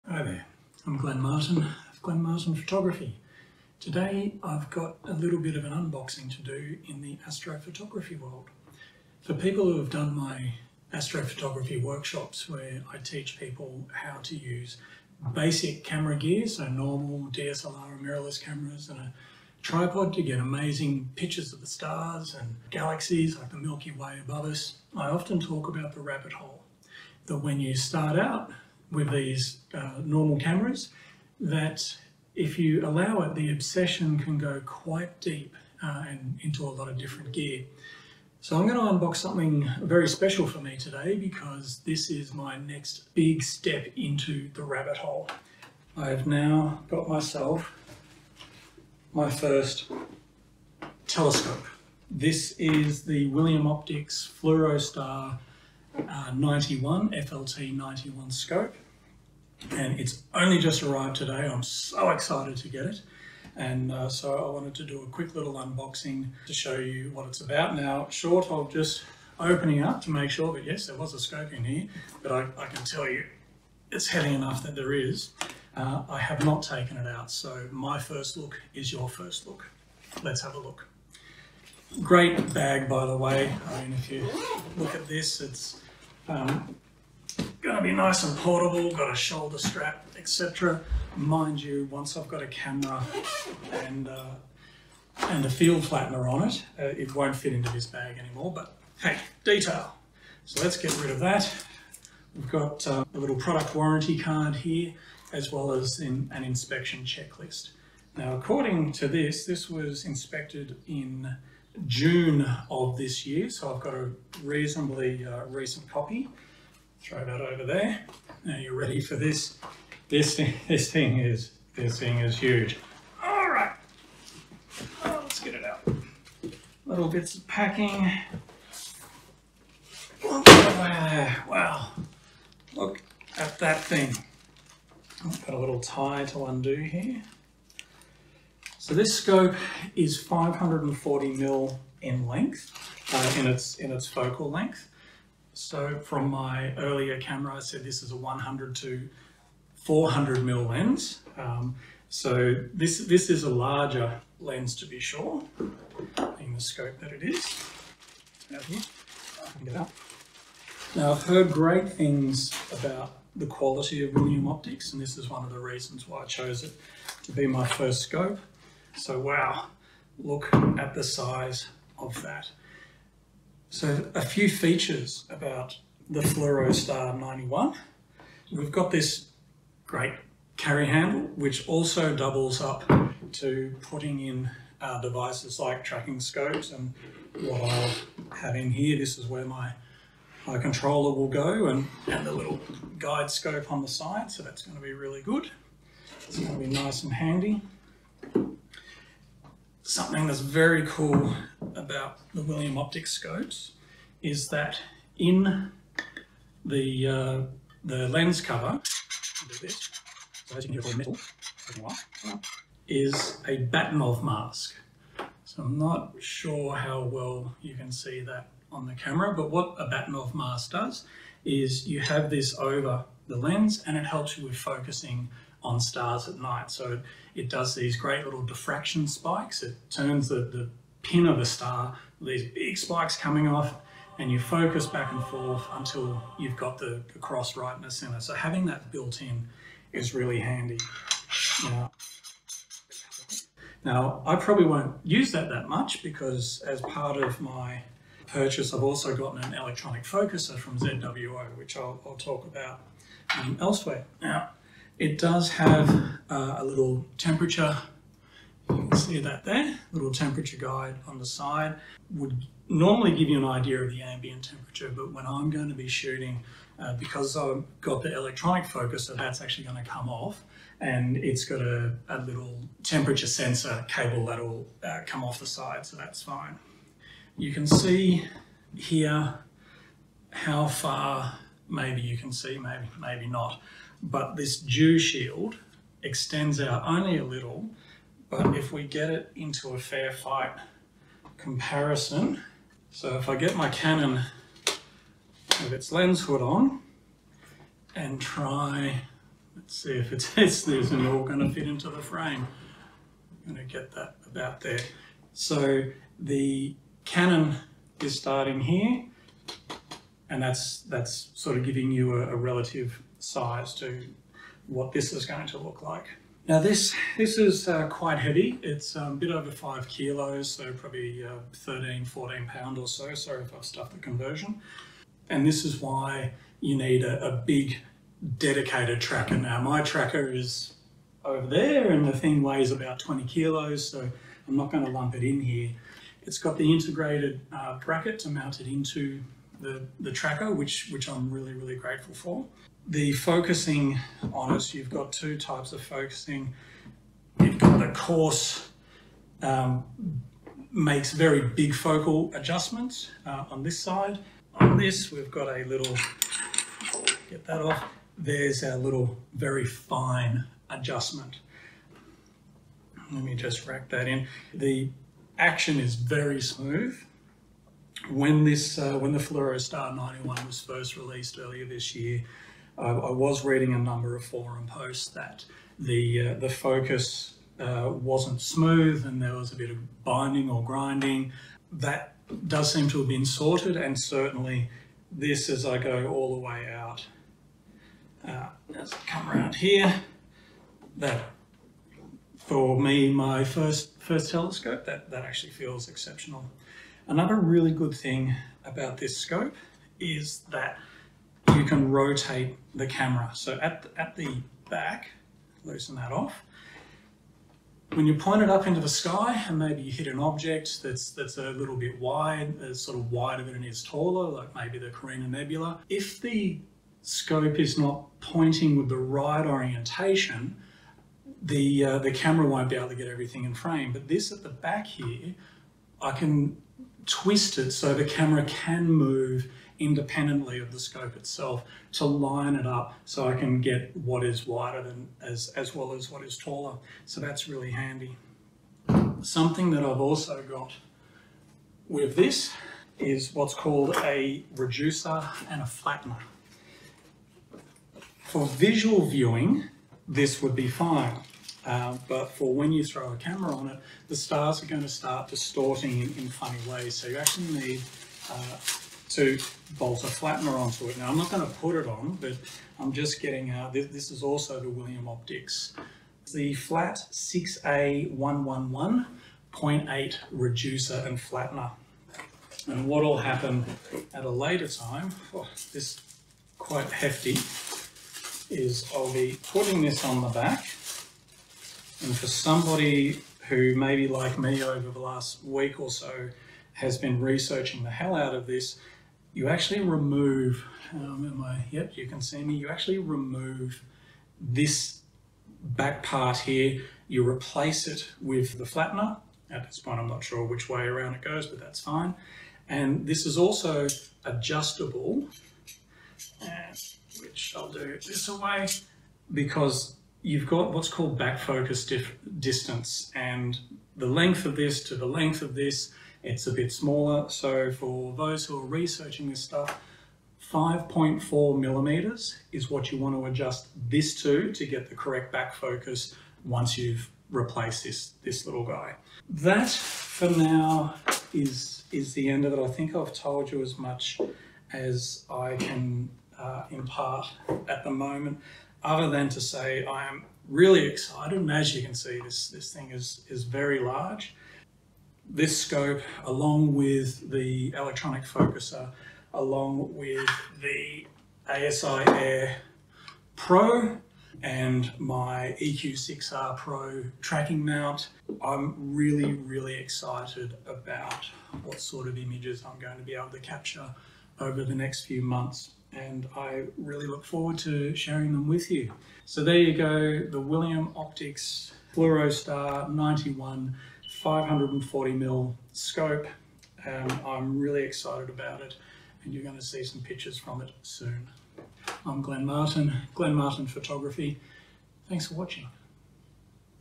The William Optics Fluorostar 91 FLT91
To see how to install the Flat6Aiii including the correct back focus go to the following link:

To see how to install a ZWO EAF electronic auto focuser to the FLT91 go to the following link:

0:00 Introduction
0:47 My new William Optics Fluorostar 91
02:53 The carry handle of the FLT91
3:18 The Bahtinov mask
04:14 The temperature gauge
04:37 The lens hood
05:15 The weight
05:35 The Vixen rail
05:42 The focusing knobs
06:36 The camera rotation/rotator
07:18 The flattener/reducer – purchased separately
08:48 The conclusion